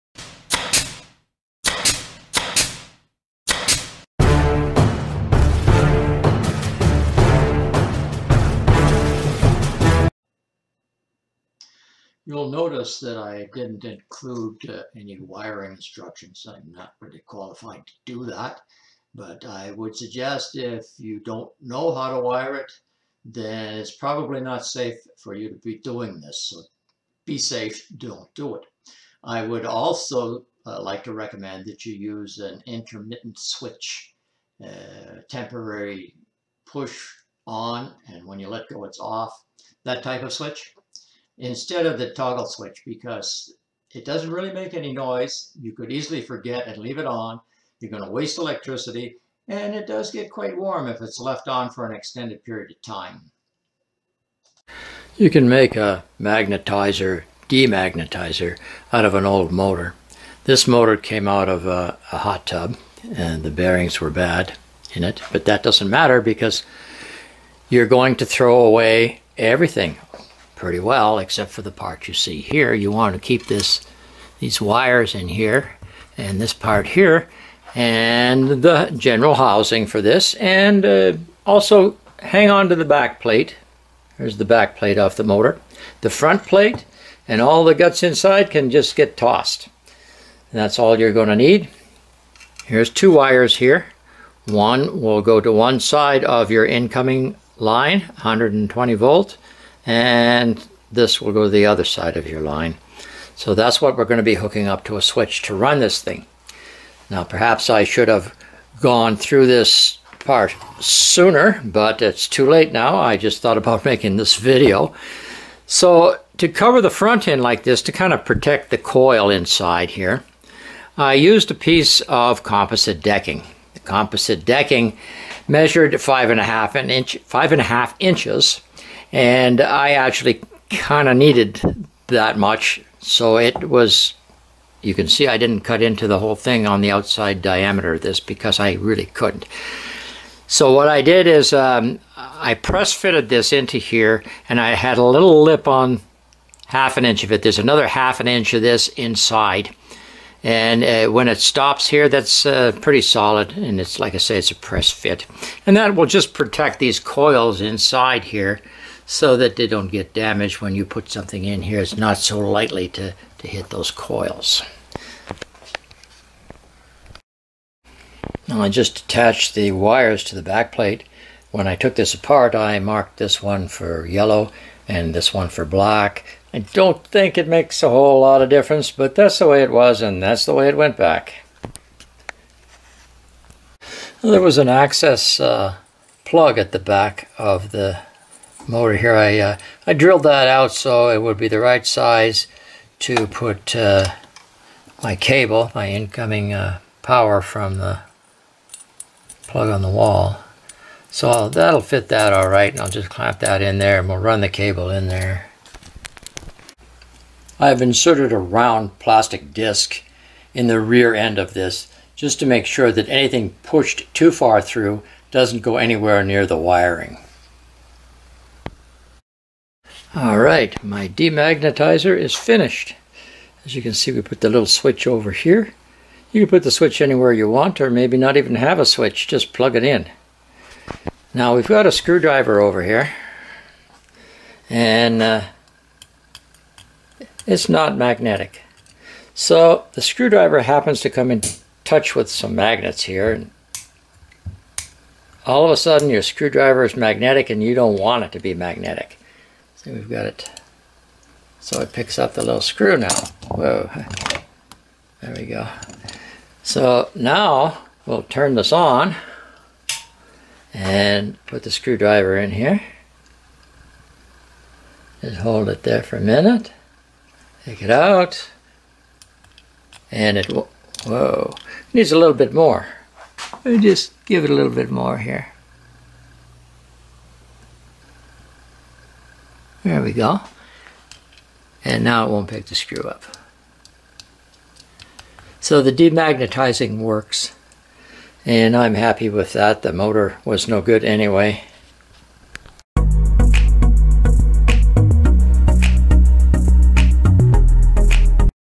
Demagnetizer
Make a demagnetizer from an old electric motor.